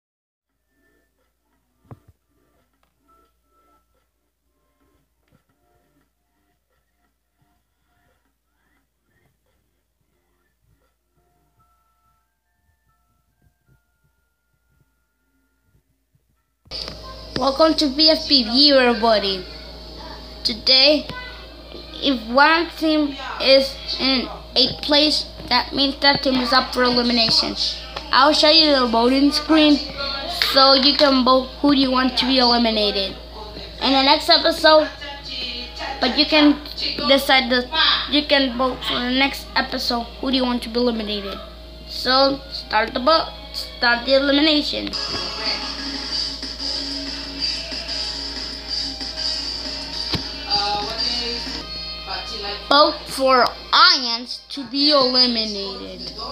Bfb viwer voting 1
Vote for snowball to be ❌

Vote for fanny to be ❌

Vote for pencil to be❌

Vote for match to be ❌

Vote for ruby to be ❌

Vote for lightning to be❌

Vote for bubble to be❌

Vote for flower to be ❌